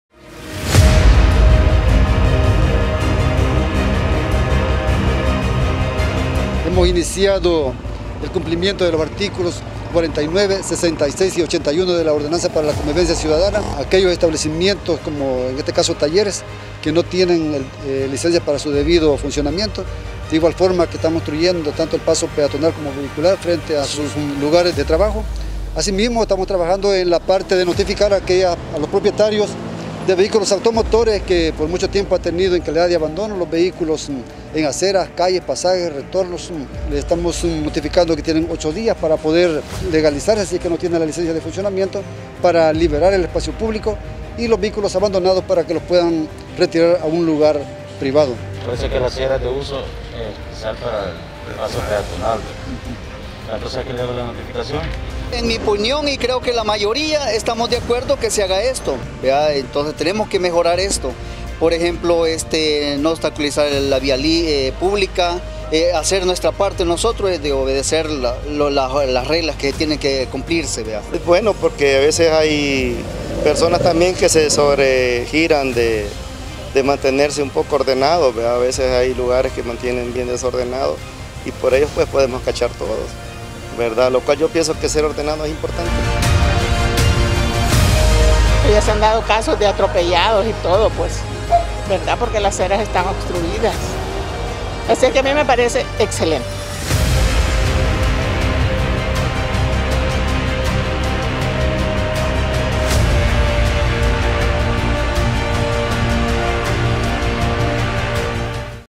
Alcaldía capitalina instó a propietarios de talleres a tramitar sus licencias de funcionamiento
Tras al anuncio del alcalde Mario Durán a los propietarios de talleres automotrices que no cuentan con permisos para operar y usurpan la vía pública, el CAM inició una jornada de notificaciones sobre el plazo que ha dispuesto la comuna capitalina para que estos negocios cumplan con las ordenanzas y el pago de tasas municipales, además deben despejar las calles y aceras de la ciudad.